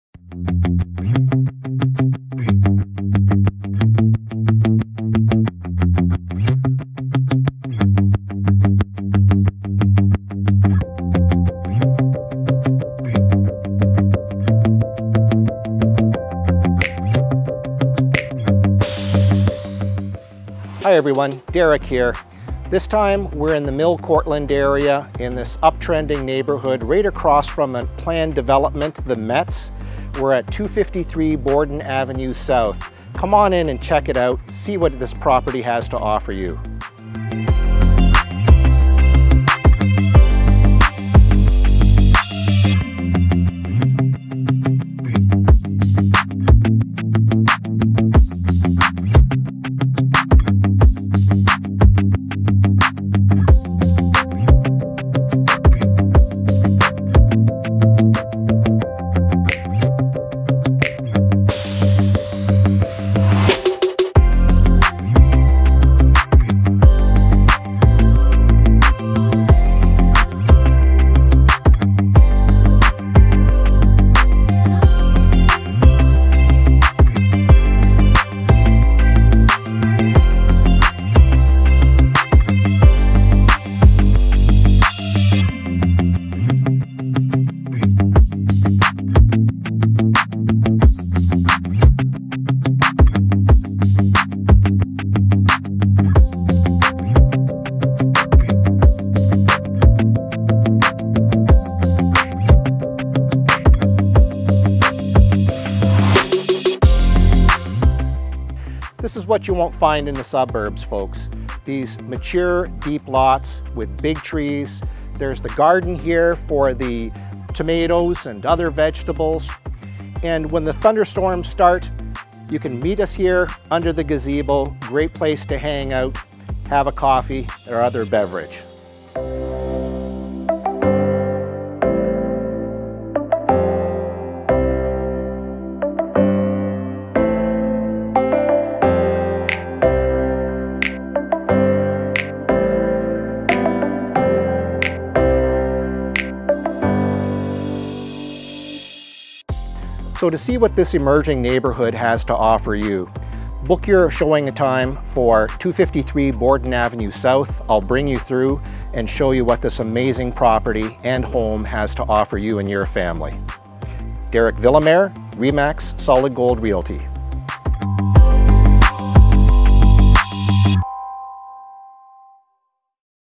253 Borden Ave. South, Kitchener, Ontario
Situated in the up-trending Mill-Courtland neighbourhood, steps to two “ION” LRT Stops, plus exciting “mixed use” redevelopment “The Metz” with planned dog park, splash pad, doctor’s office, grocery store, and urban plaza directly across the way, 253 Borden Ave. South is poised for a bright future! Newly Renovated Main Bath with gleaming white subway-style tile and all new fixtures--bright and fresh.  All new vinyl plank in neutral tone just installed on Upper Level in this Carpet-Free home. The generous space in the Master Bedroom is complemented by a large closet (was two bedrooms and could be converted back to a 3 bedroom home).  The Family room addition featuring new laminate is bright and cozy, with sliding patio door to yard, and handy two piece powder room just around the corner. The Dining Room is a great space for entertaining and doing activities. Classic white cabinets in the Kitchen with island & breakfast bar, creates a relaxed space to prepare anything from cakes to crab…or crabcakes! A sunny front porch room is enclosed and just right for your herbs and houseplants, or just chilling with a book. Let the pets and kids run wild in the fully fenced yard, both deep and shaded with mature trees. Potential to build a rooftop deck or second story over the rear family room addition (Buyer to satisfy their own due diligence). Garage recently re-wired, new insulation and roof 2018. Water Heater 2018. Roof 2016. Updated Plumbing. Rough-in for bathroom at basement level. Not far from Expressway, Parks, DT Kitchener, The Aud, Farmer’s Market, Golf, YMCA, and Shopping.

MLS 30827154